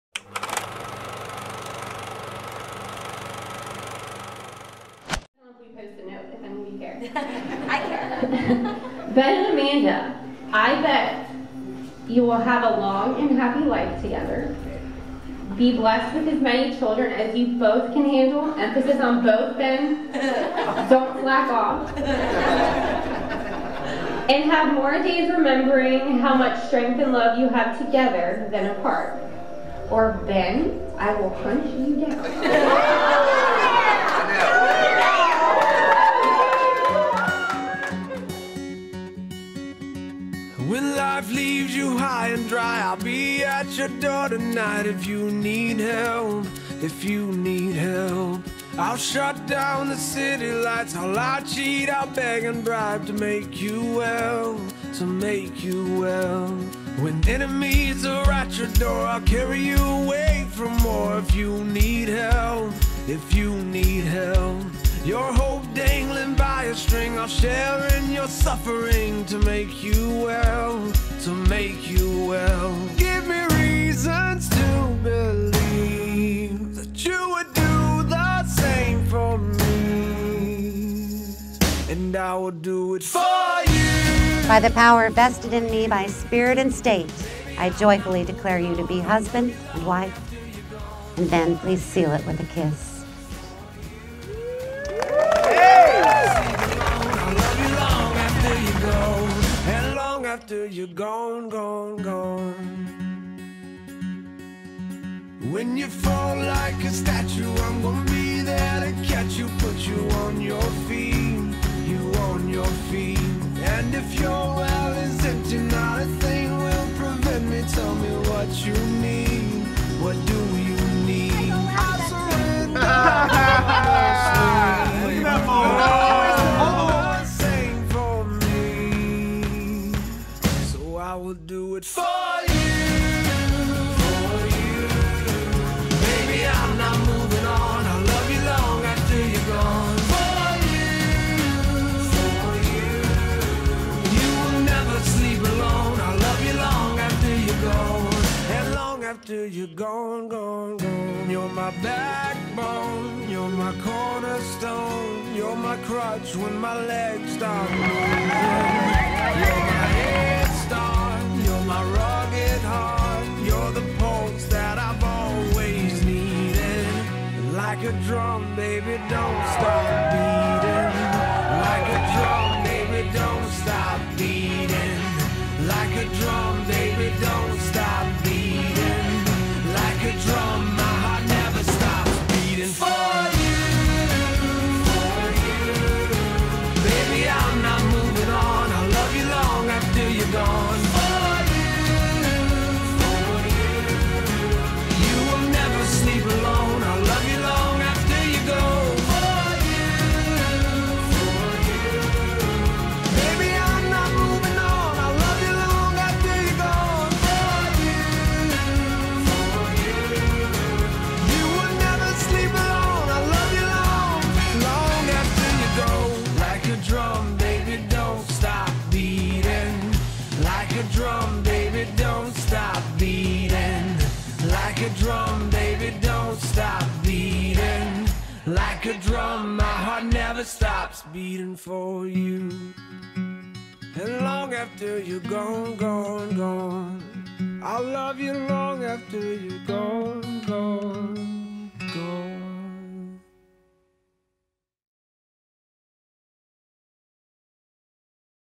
The Franceschi Wedding Trailer
Outer Banks wedding videography by Reel Love Weddings. Ben and Amanda's Corolla wedding was another one for the books. The sun was shining over their ceremony out on the peninsula at The Whalehead Club, and the historic Currituck Beach Light added to the backdrop. Rev. Shirley Anne officiated the ceremony and Marie Russell coordinated the whole day wonderfully. Filming a wedding is always fun, especially when the reception party is as awesome as this. Another Level played live music all night and kept the crowd engaged and dancing! We worked alongside Amanda Hedgepeth, who was capturing some amazing photos as well.